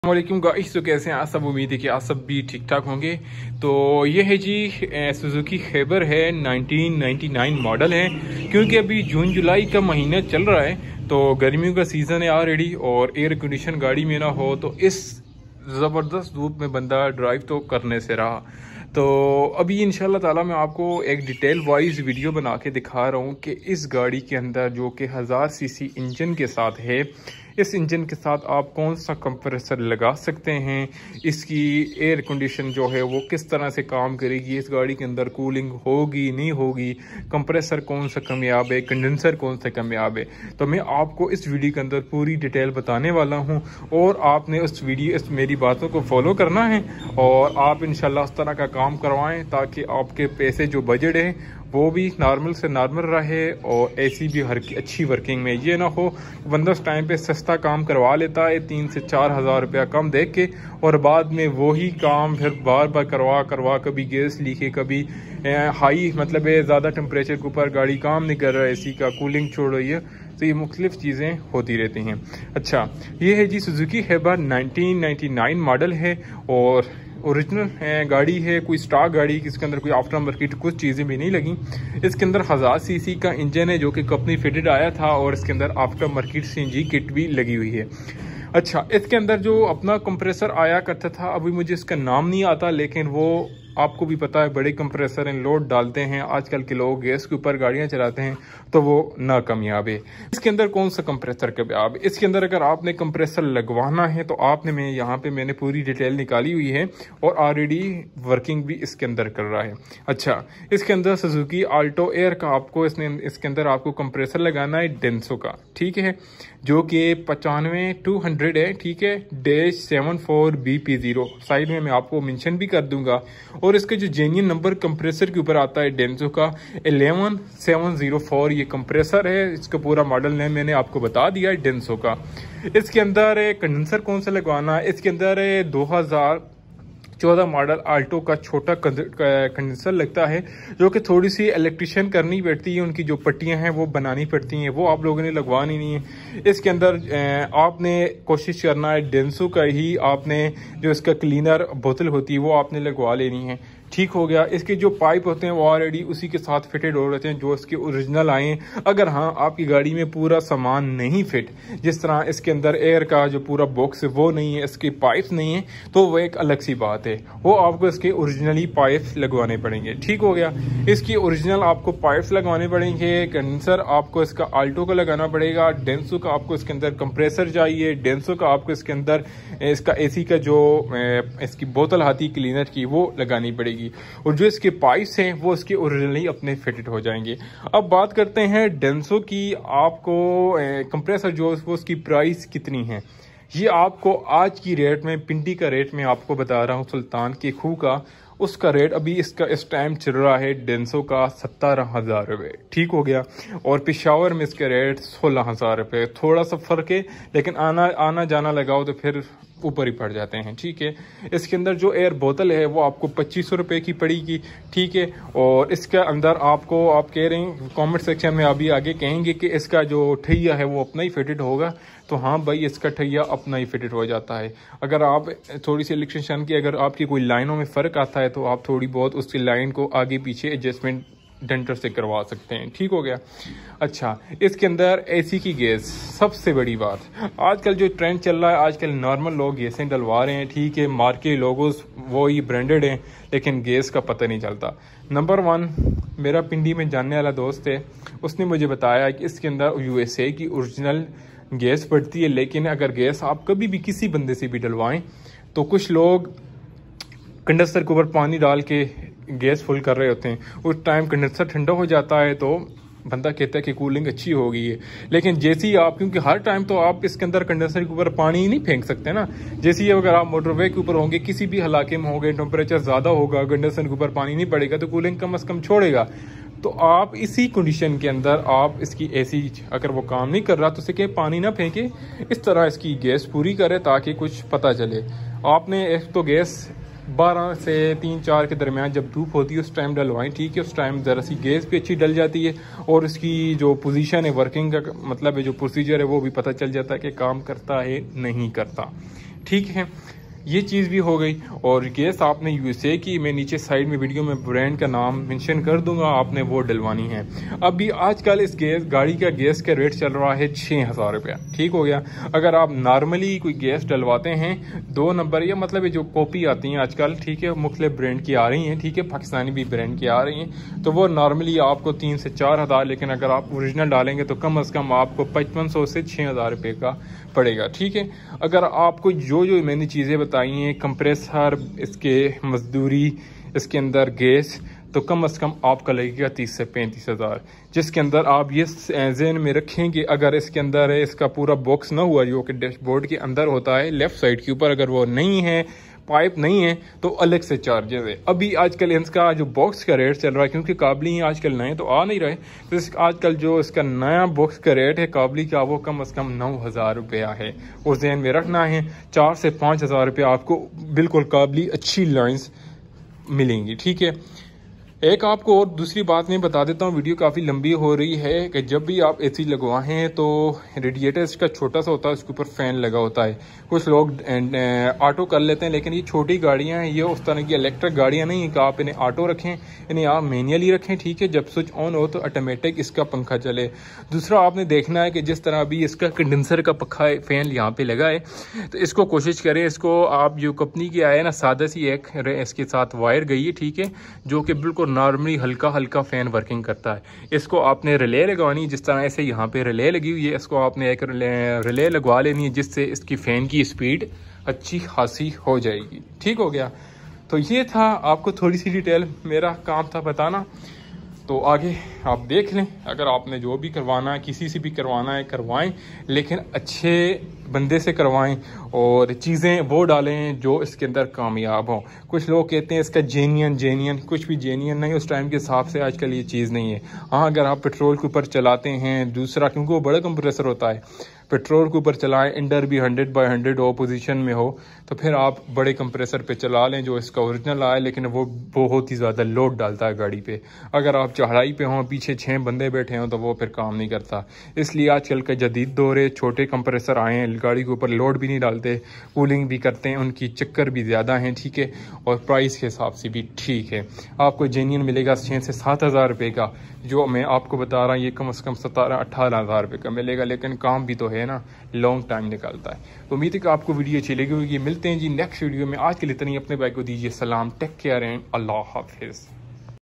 Suzuki Khyber 1999 Complete AC Installation & Performance Test
Please provide us with your valuable feedback.